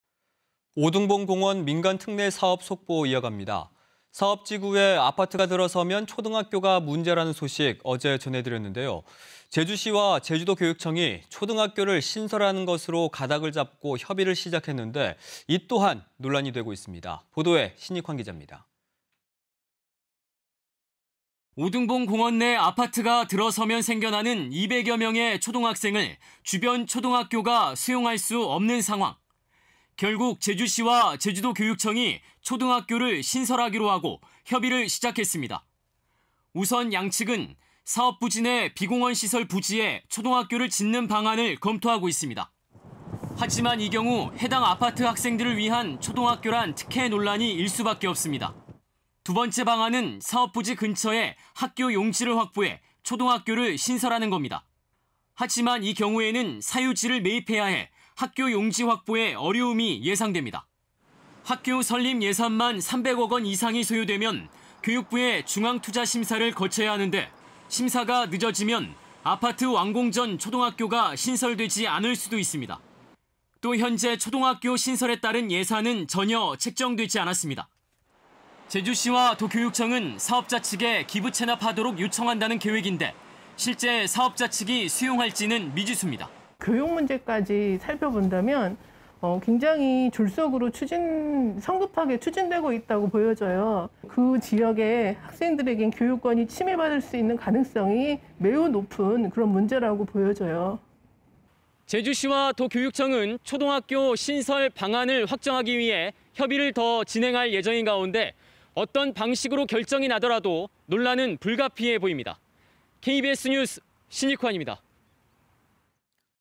[9시뉴스제주/주요뉴스] 오등봉공원 초등학교 신설 가닥…특혜 논란 ‘불가피’ / KBS 2021.04.09.
오등봉공원 민간특례사업으로 아파트가 들어서면 초등학교가 문제라는 소식, 어제 전해드렸는데요.
제주시와 제주도교육청이 초등학교를 신설하는 것으로 가닥을 잡고 협의를 시작했는데, 이 또한 논란이 되고 있습니다.